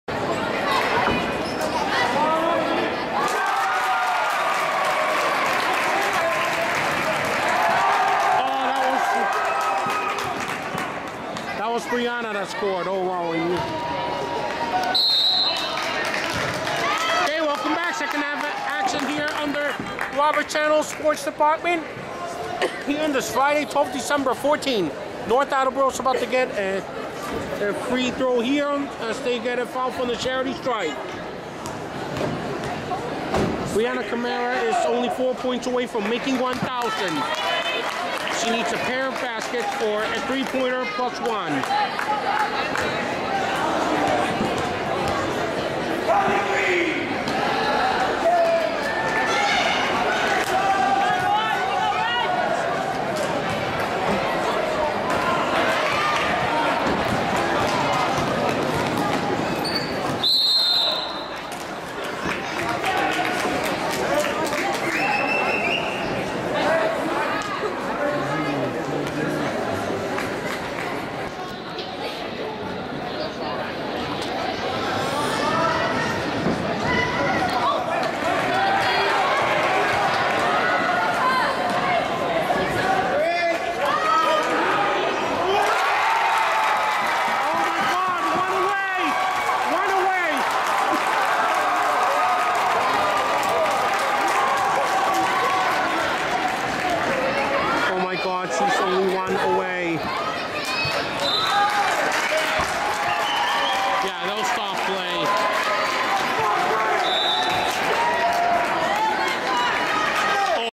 North Attleboro at Durfee Girls Basketball game played on 12/12/14
Highlights of the North Attleboro girls basketball game played at Skip Karam Gym at BMC Durfee High School on 12/12/14. Durfee won 57-52